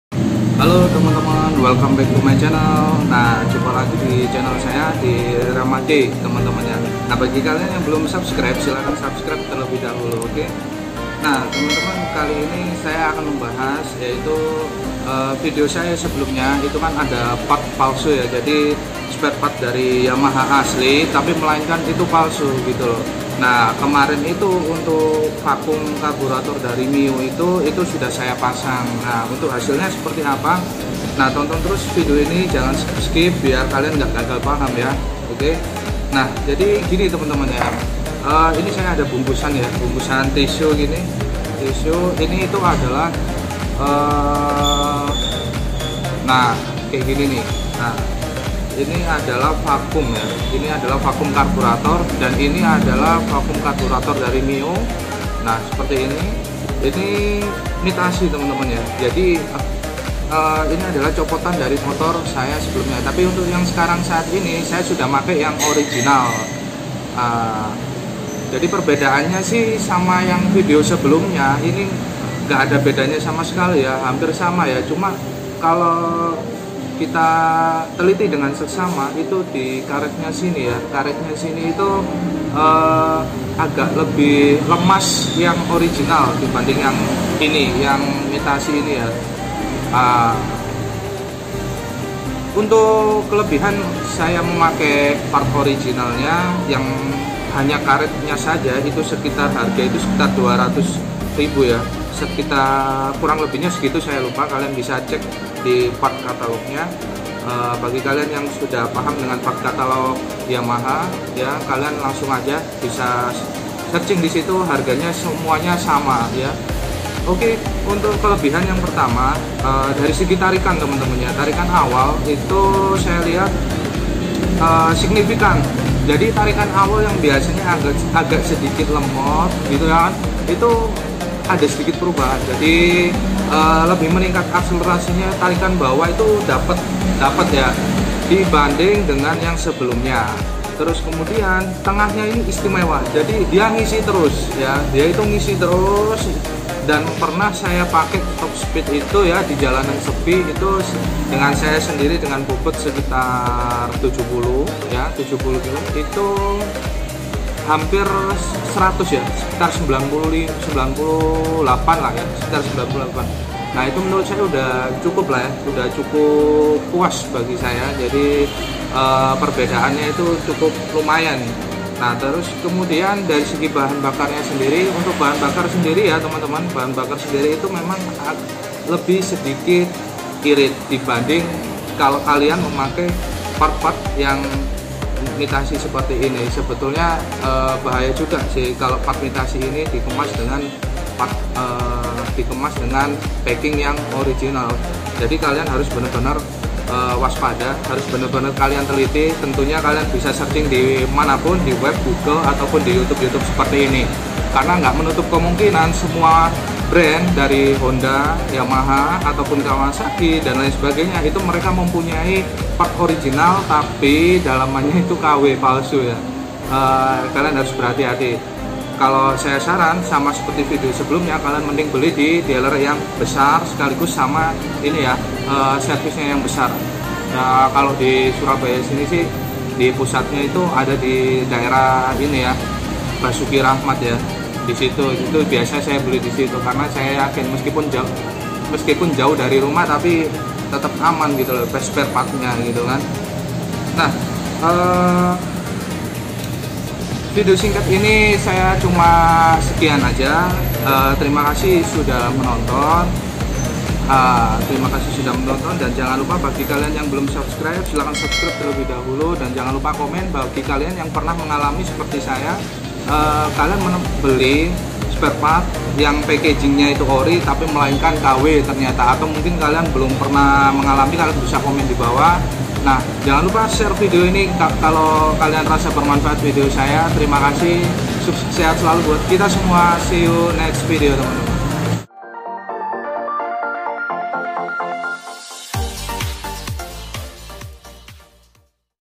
Sparepart yamaha PALSU
Disini saya memberikan info sparepart yamaha palsu dan juga sangat berbahaya apabila spare part palsu beredar tidak menutup kemungkinan honda yamaha kawasaki dan lainnya dipalsukan oleh pihak yang tak bertanggung jawab
Comment jika kalian menemukan spare part yamaha palsu atau spare part honda palsu dan jika kalian bisa membedakan spare part yamaha atau honda palsu kalian bisa comment dan share.

Cara membedakan spare part palsu
cara membedakan spare part yamaha asli dan palsu
spare part honda asli dan palsu